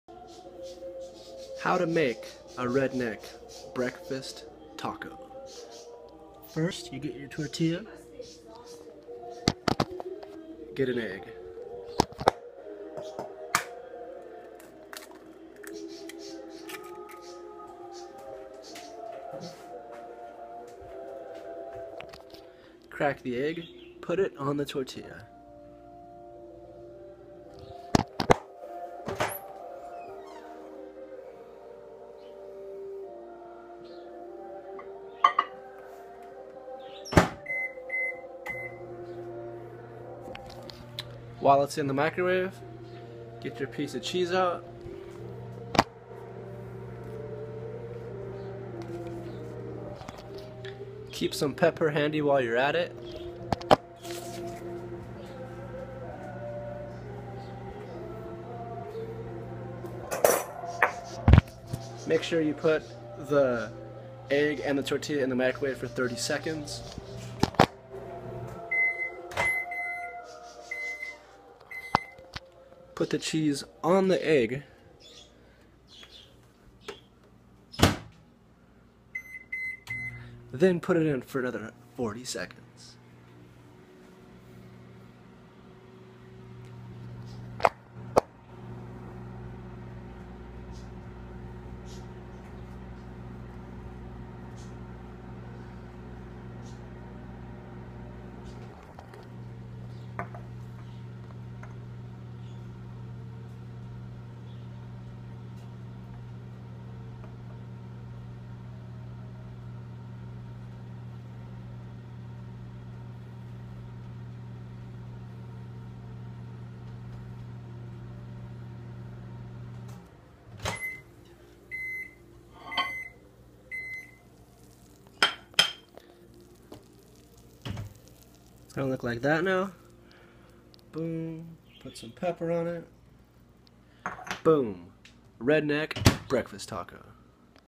Redneck Breakfast Taco
A quick way to make a redneck breakfast taco
Ingredients
Tortilla
Egg
Cheese
Pepper